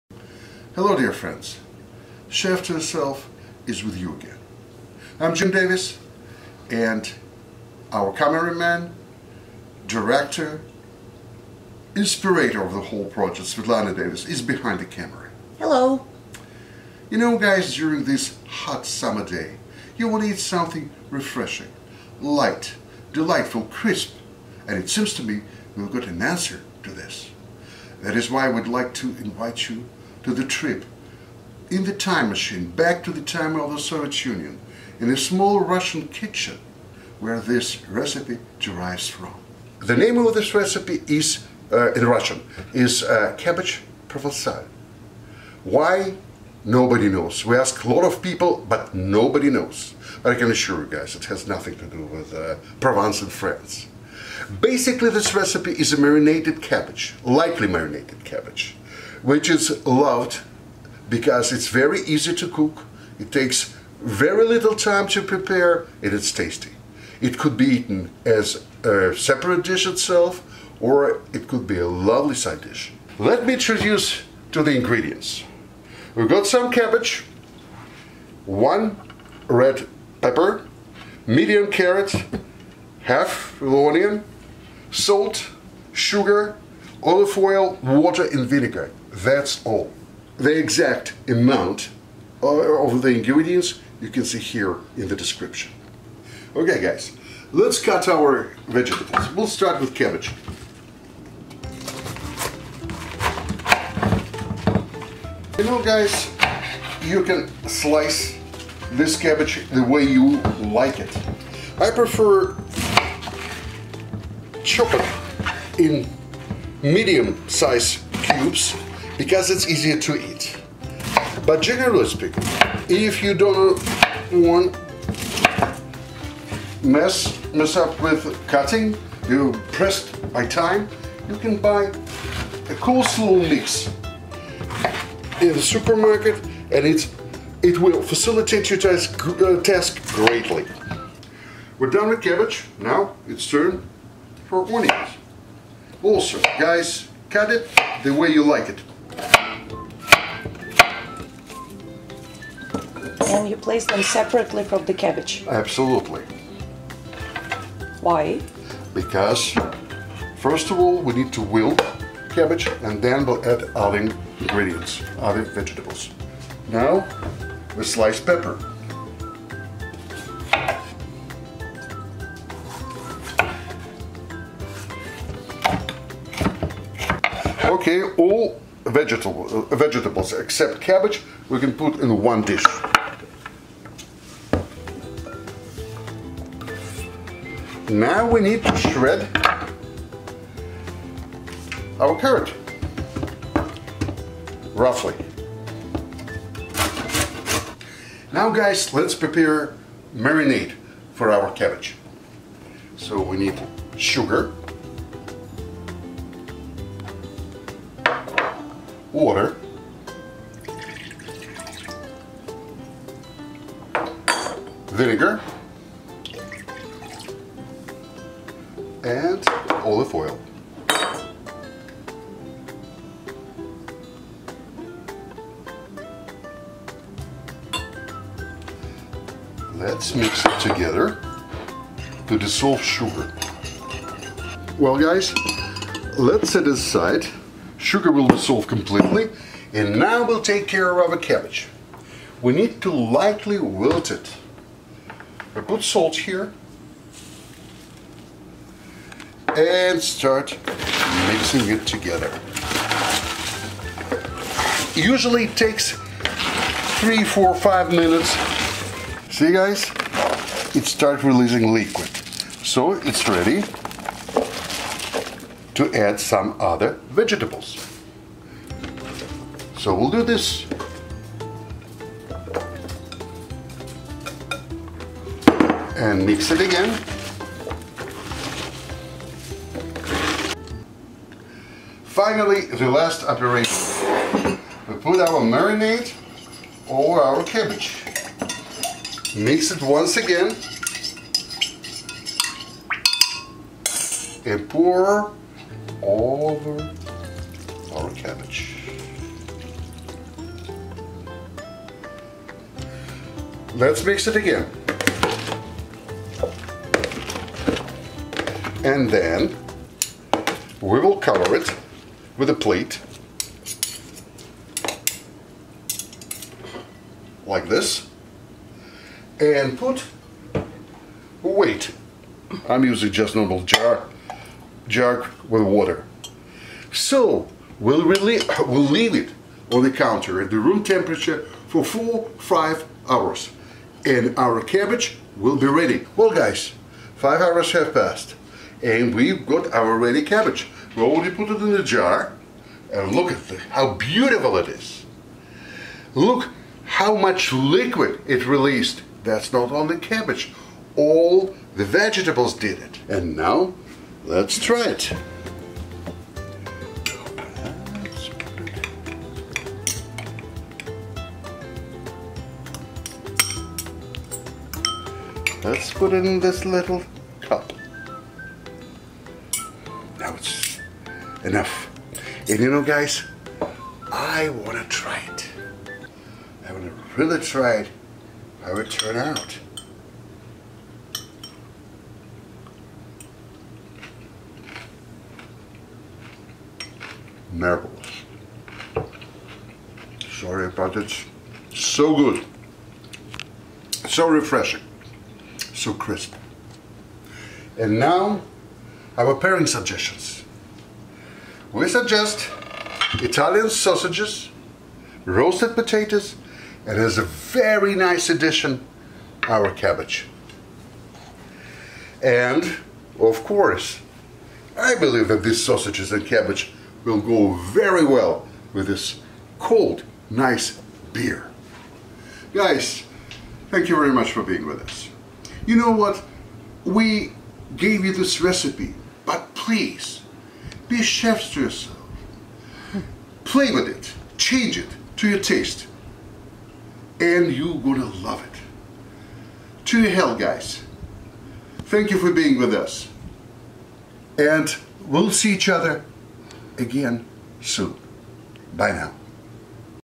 Marinated Cabbage
Marinated Cabbage Russian style – light, elegant, crisp and juicy side dish or a salad base – what else could be better for you table in the summer heat! This dish can easily replace heavier coleslaw or be used in tacos; can be served as a side dish, be an essential component in light summer salads or just be eaten by itself. What is the most important – it is very easy to cook and not time-consuming at all! Try it – and you’ll fall in love!